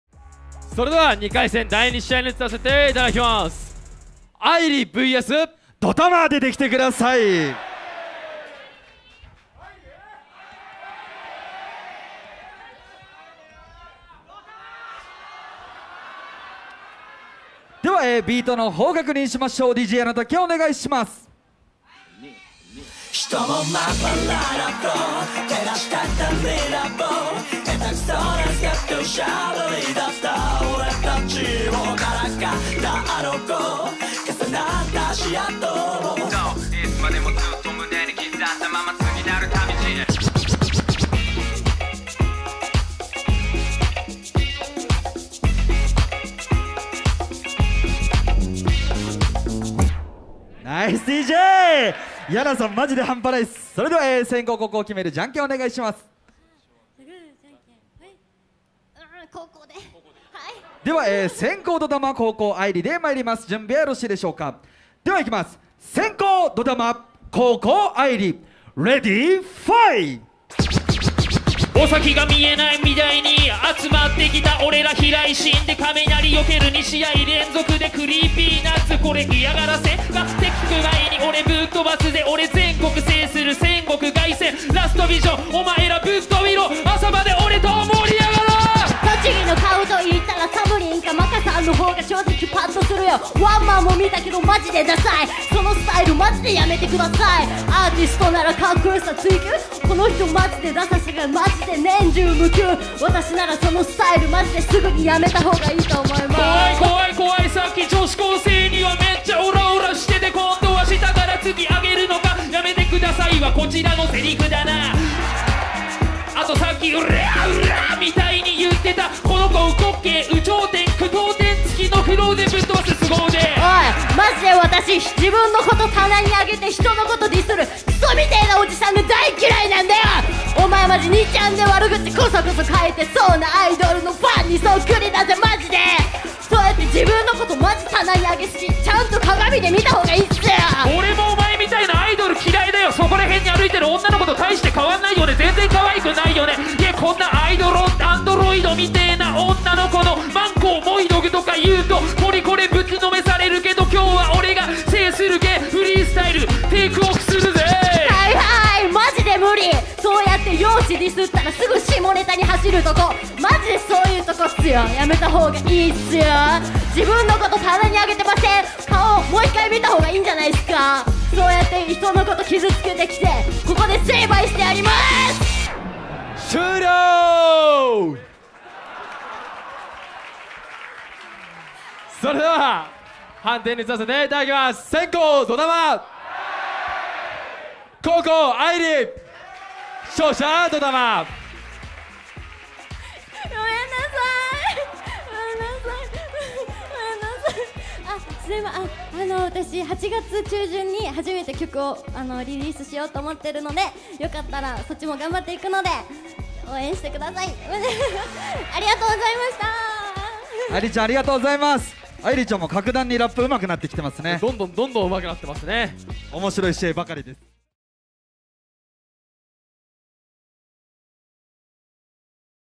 DOTAMA vs AILI a.k.a 立仙愛理/戦極vs凱旋 MCBATTLE LAST VISION(2022.7.17)
▼フルLIVEの視聴はこちら

2大MCバトル大会である”戦極MCBATTLE”と”凱旋MC Battle”が
大会のプライドを賭けて激突!

今回は、9月に閉店する渋谷・道玄坂のクラブ「SOUND MUSEUM

VISION」で最後の開催!!

2022.7月17日
戦極vs凱旋 MCBATTLE LAST VISION

GUEST LIVE
CYBER RUI

MC BATTLE
1百足
2喉電波
3Itaq
4紺野ぶるま
5SONOTA
6DOTAMA
7ふぁんく
8RAWAXXX
9AILI a.k.a 立仙愛理
10Nidra Assassin
11SURIBOY
12KT
13TERU
14脱走
15L-指定
16Fuma no KTR

DJ
YANATAKE/MASTERKEY

HOST
MAKA/ONJ